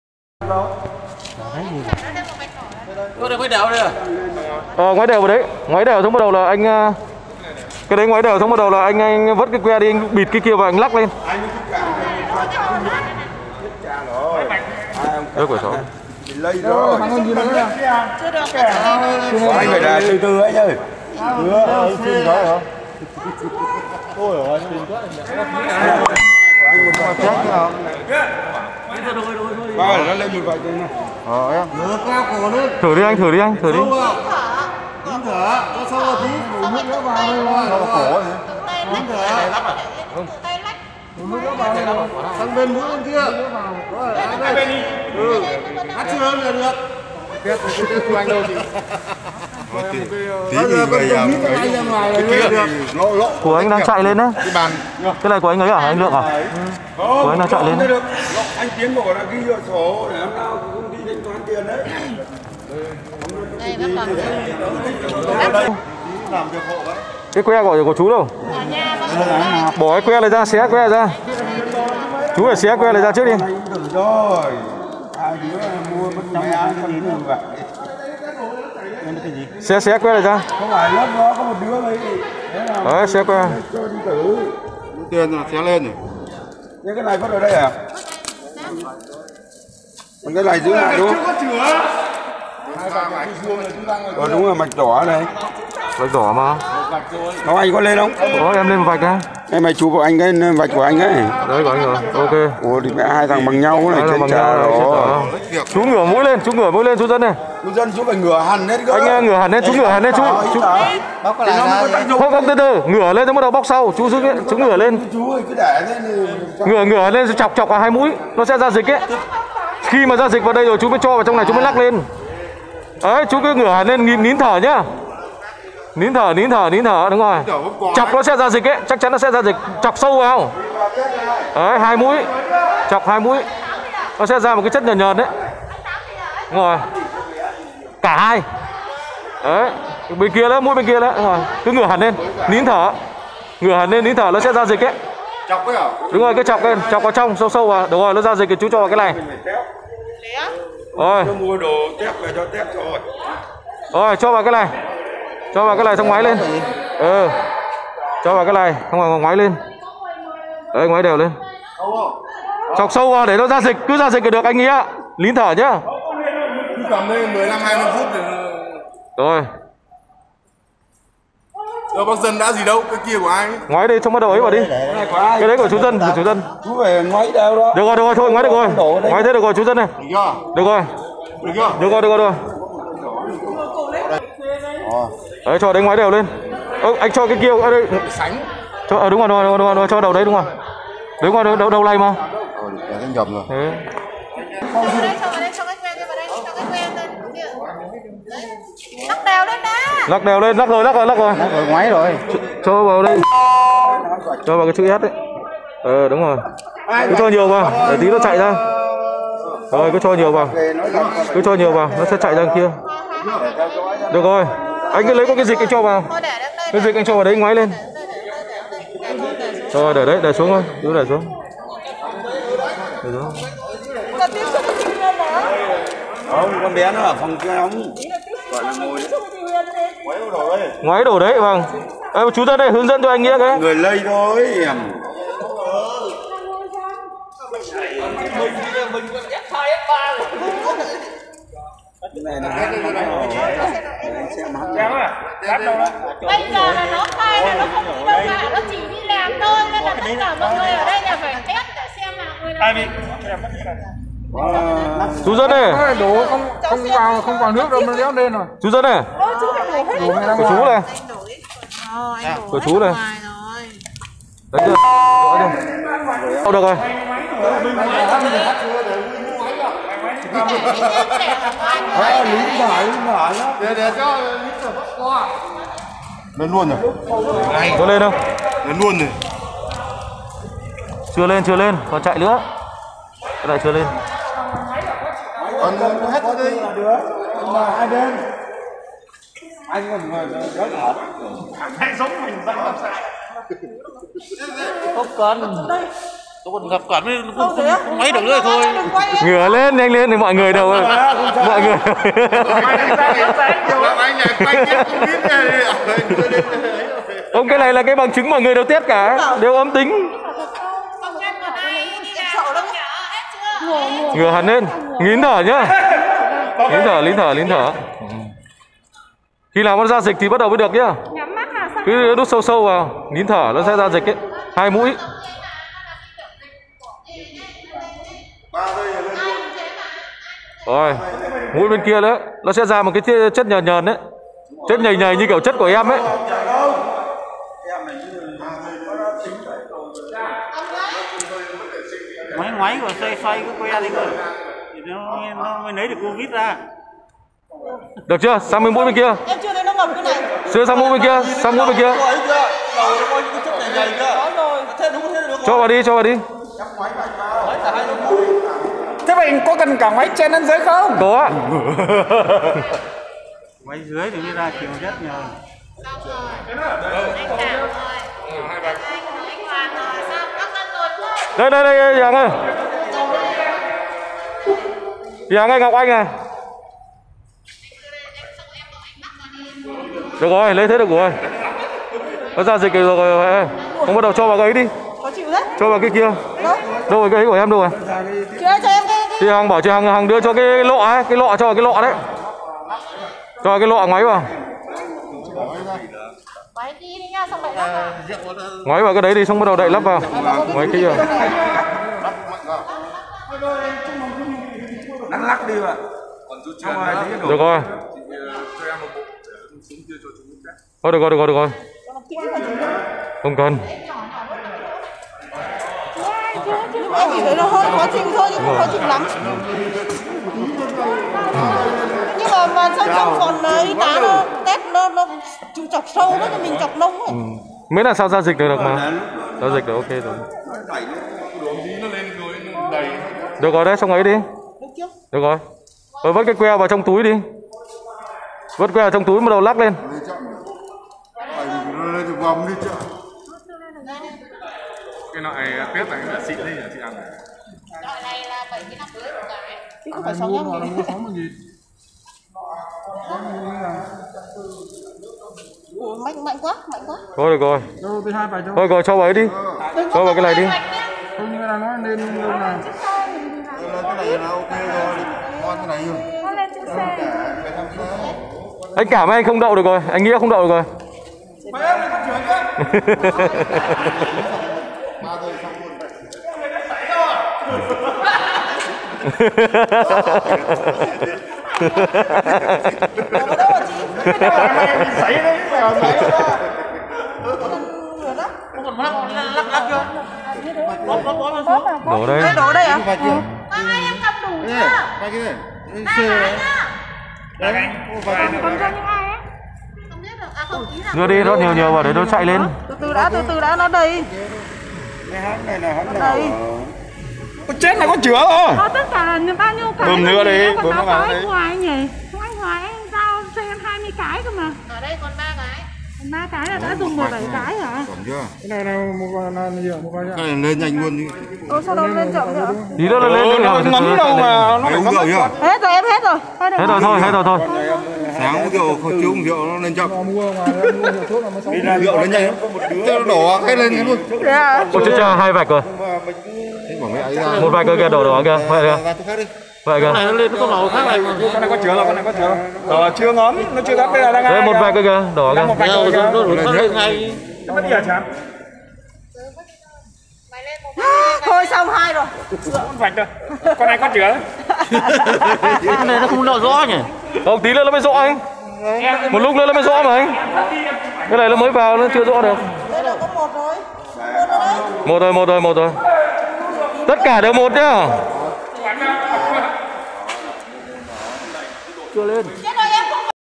Date: 19/01/2022, Test Cô Ấy tại kho CFS | Hải Phòng Logistics.
Test Cô Ấy tại kho CFS, tất cả đều cười tươi vì Cô Ấy không yêu mình | Hải Phòng Logistics.